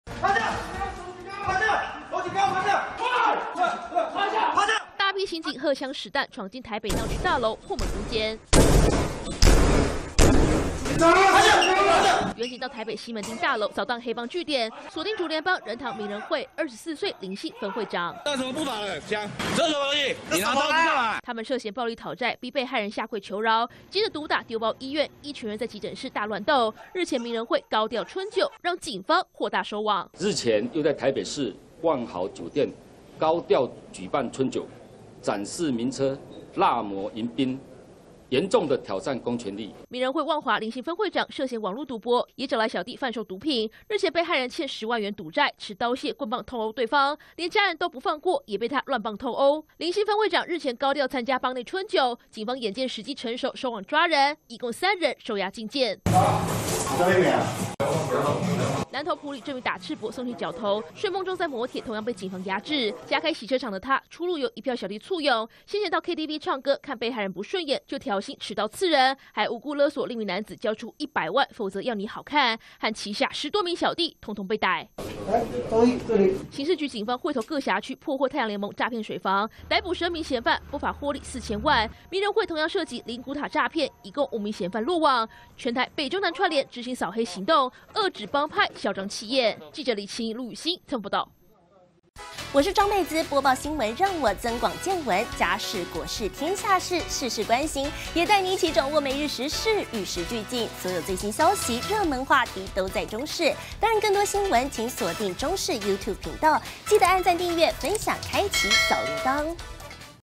高調春酒後慘了! 明仁會遭強力掃蕩"分會長落網"│中視新聞 20230317
有黑幫接連高調辦春酒，讓警方顏面掃地。刑事局全台大掃黑，查獲竹聯幫明仁會林姓分會長，涉嫌暴力討債逼被害人下跪，把人毒打丟包醫院急診室，遭裁定收押禁見；南投也有位宋姓角頭，出入由小弟簇擁，犯下多起勒贖案，睡夢中落網。